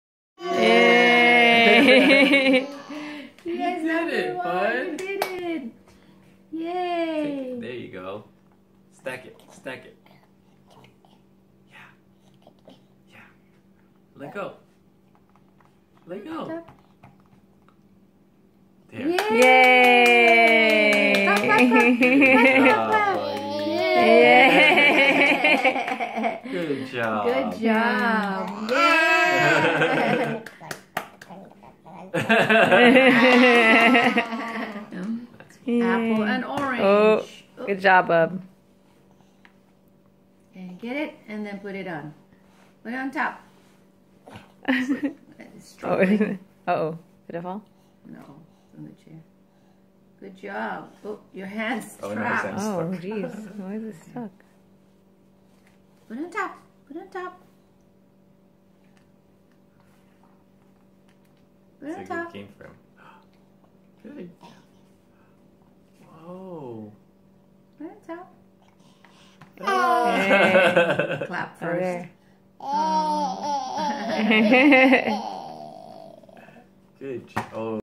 One-year old wonderments
Nolan stacks fruits!